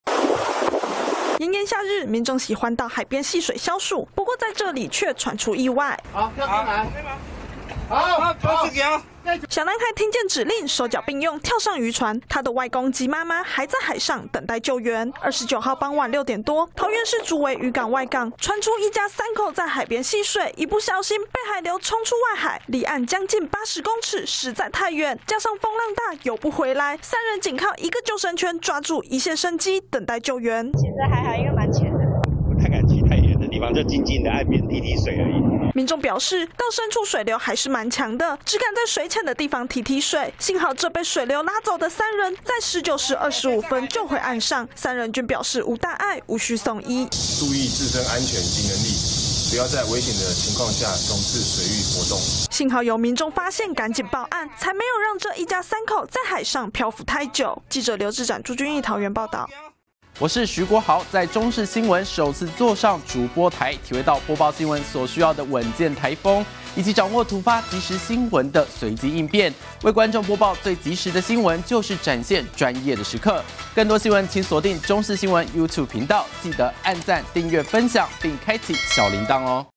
漁港驚傳戲水遭漂流出海 全家緊拉1個泳圈保3命│中視新聞 20240630
夏天又到了海邊戲水的時間!不過大家到海邊戲水要注意安全了!桃園發生一家三人不慎被水流沖走，距離太遠游不回來，只能緊抓1救生圈等待救援，所幸消防與漁民一起搶救，最後三人都沒有受傷。